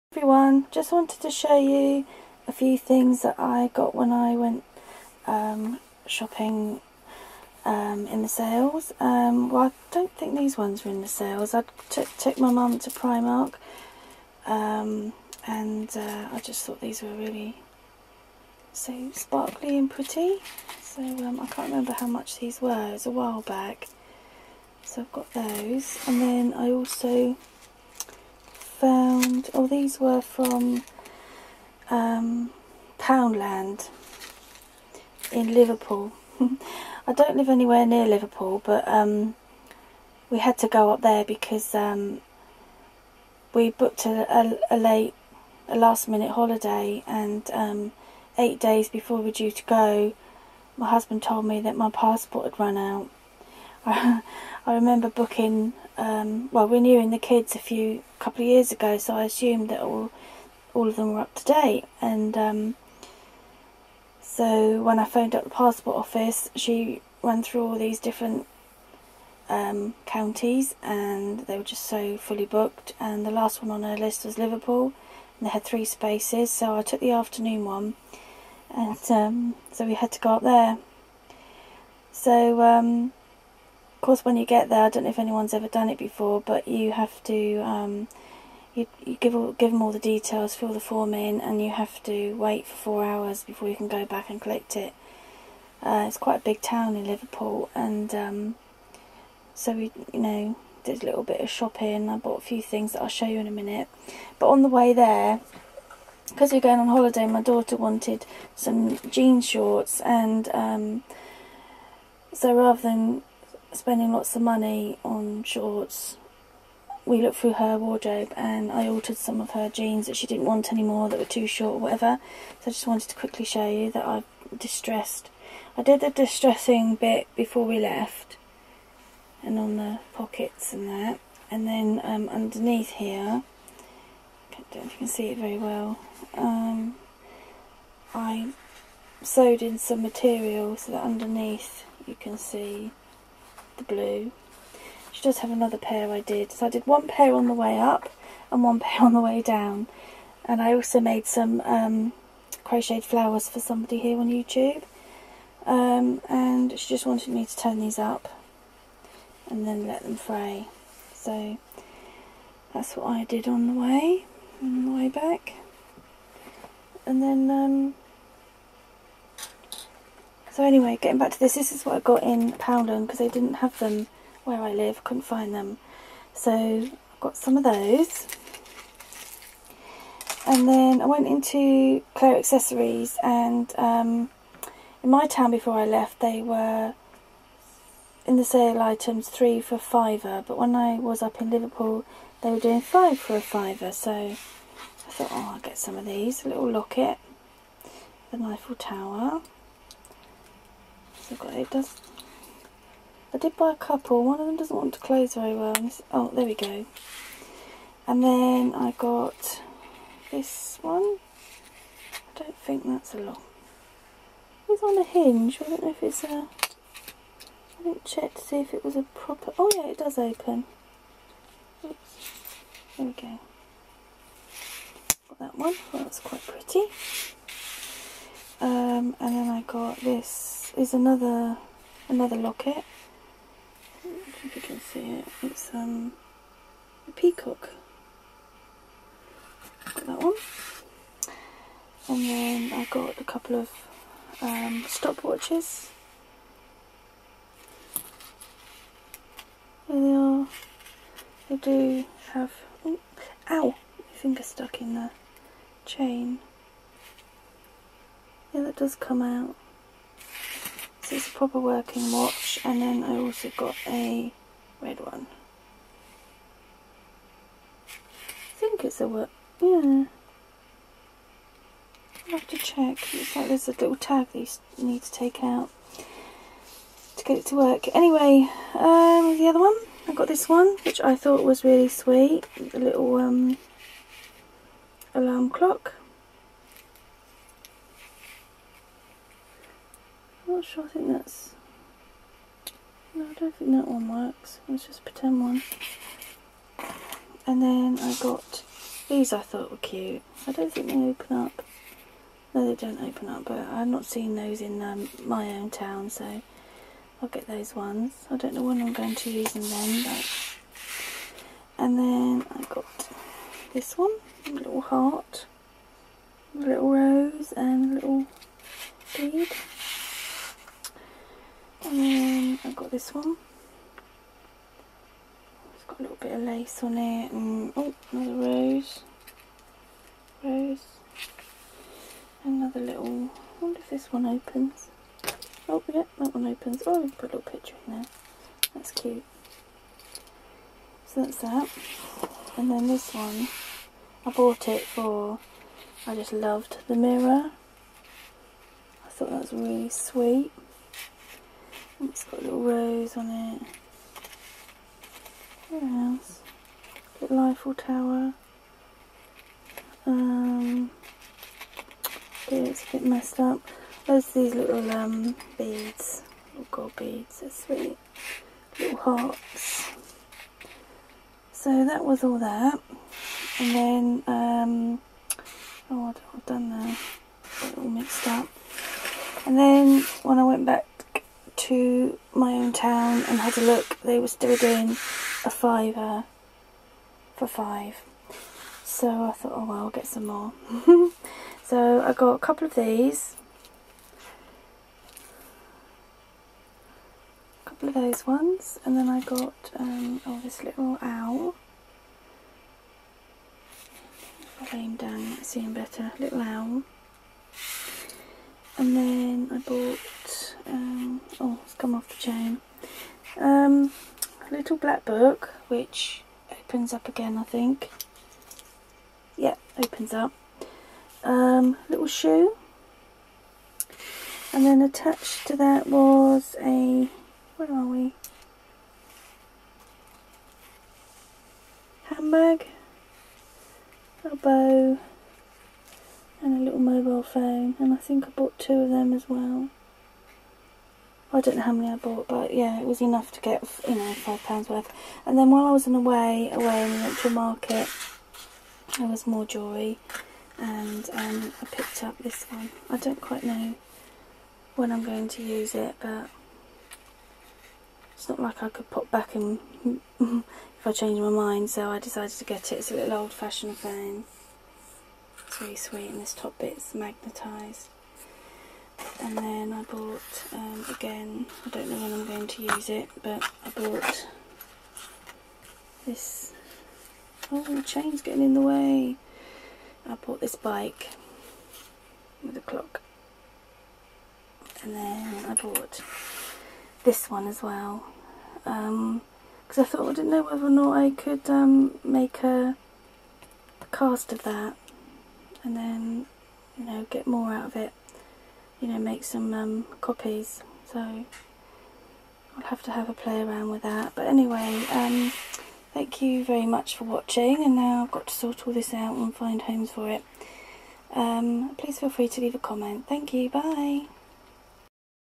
Collective jewelry hauls/jeans made into distressed shorts
Here are some pieces of jewelry picked up from several different shops over the last few weeks.  Also a pair of jeans cut down and distressed into shorts.